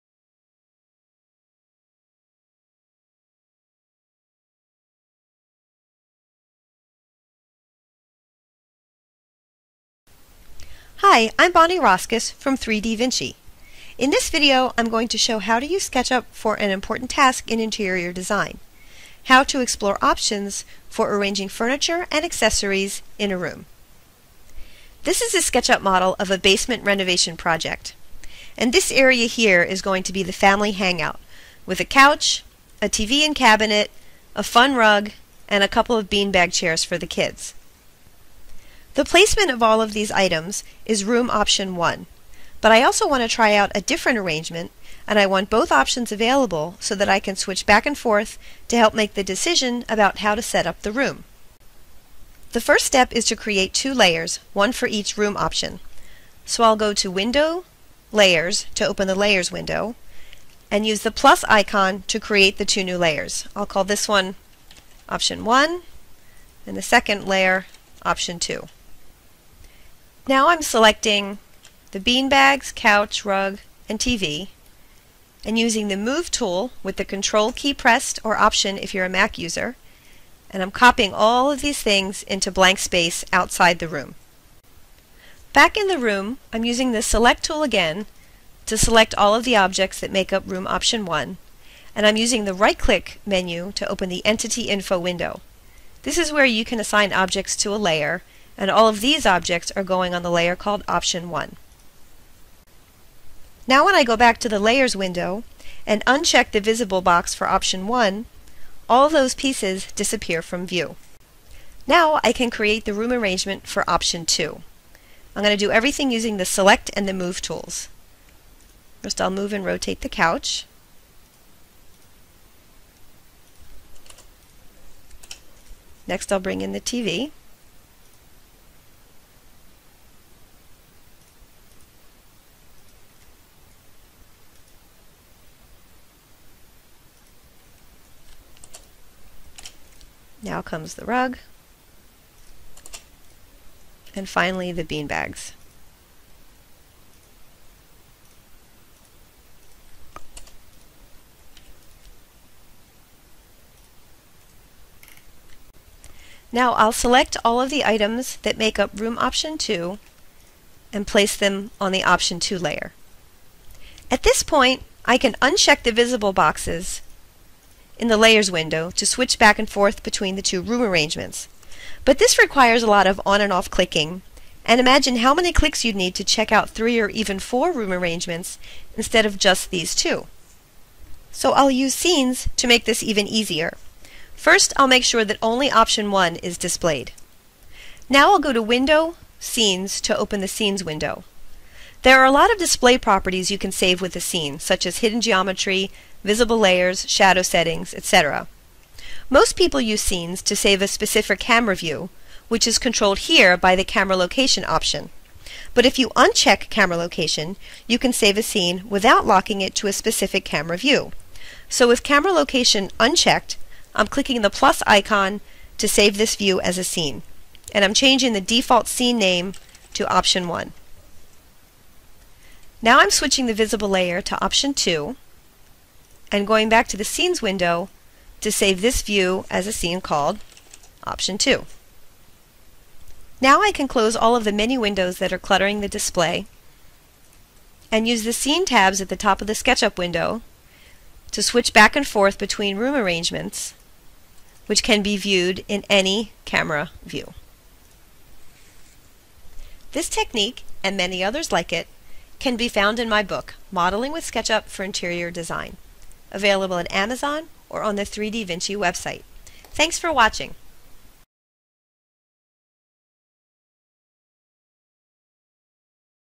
Using Scenes and Layers to Explore Room Design Options
This video shows how to combine layers and scenes, in order to explore different ways of arranging furniture and accessories in a room. The technique comes from the book "Modeling with SketchUp for Interior Design."